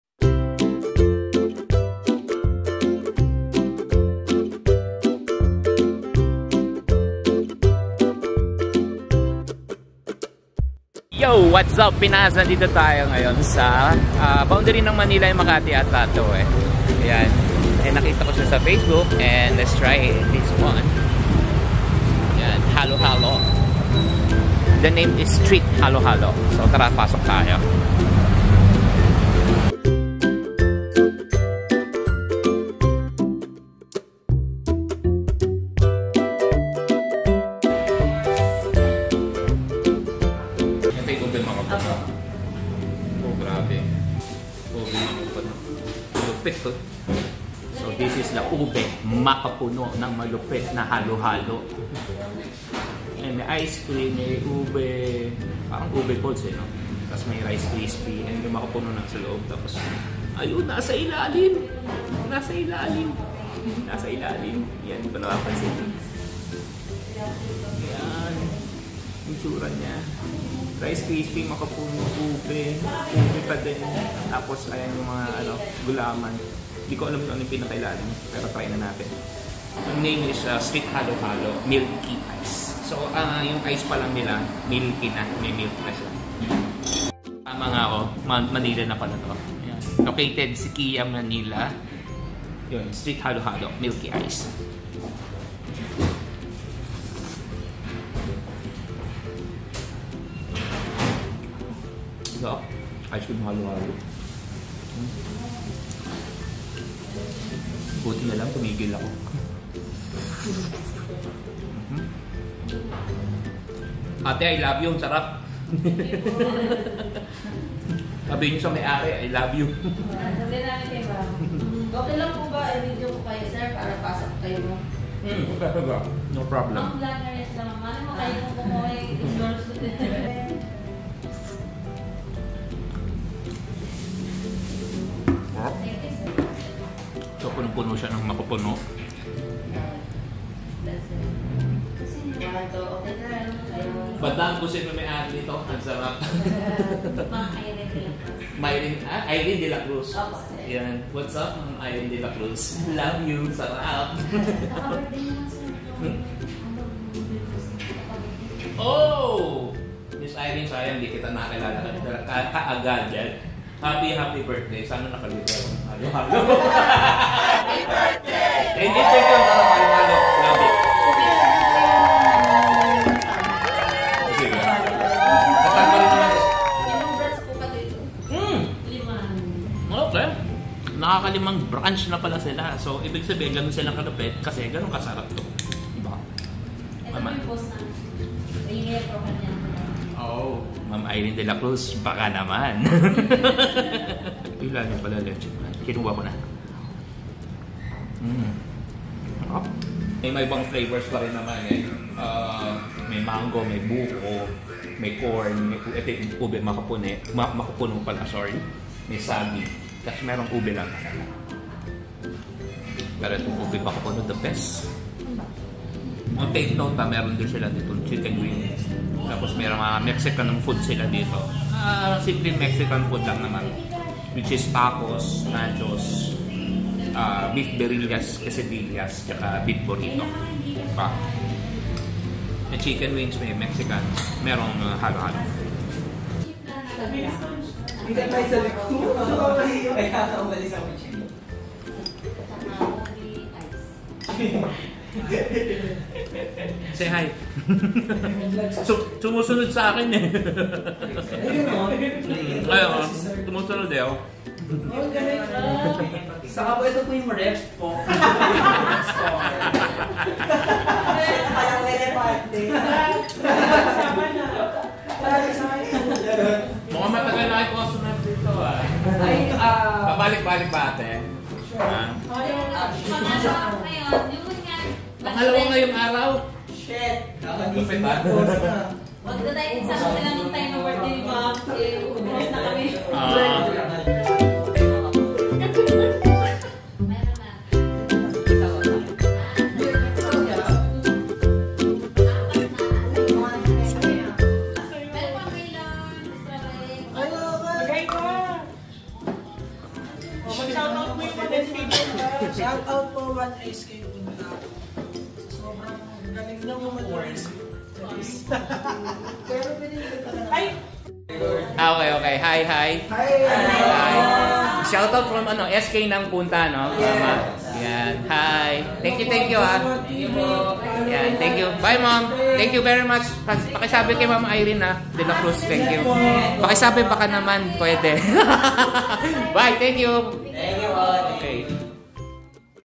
STREET HALO HALO MILKY ICE
One of the best halo halo dito sa syquia st. sta. ana manila and they got 5 branches na,